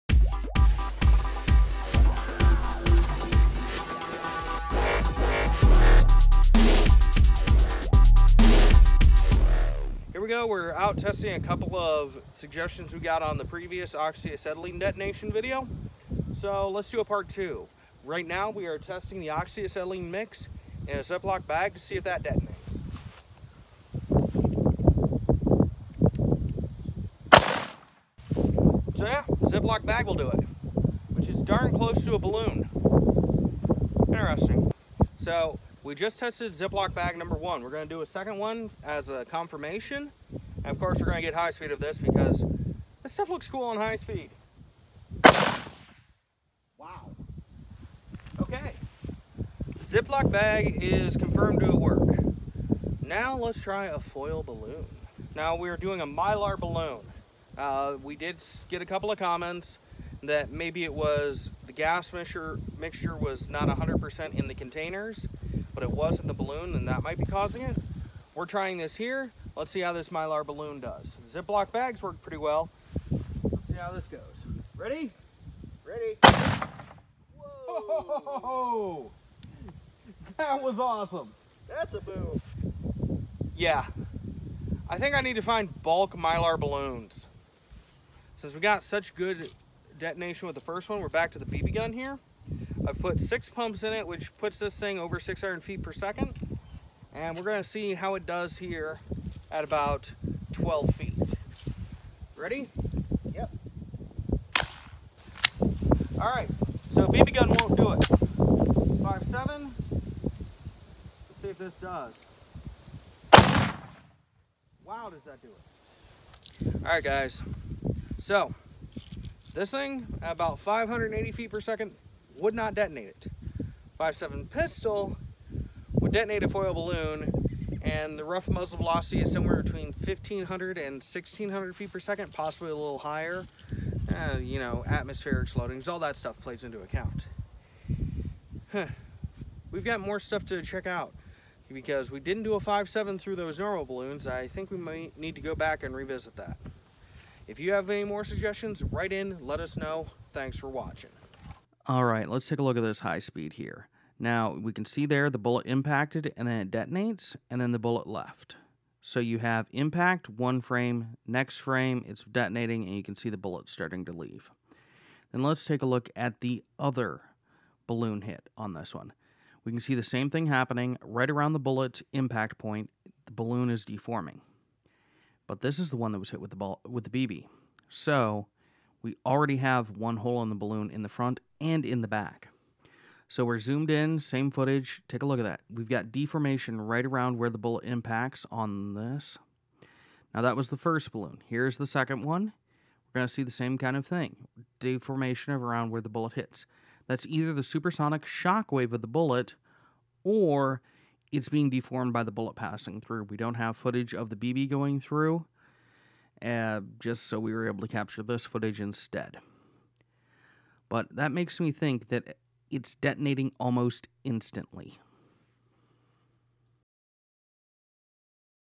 Oxy Acetylene Detonation 2
We take a look at some of the suggestions in the last video to see if we can figure out exactly what is causing the detonation.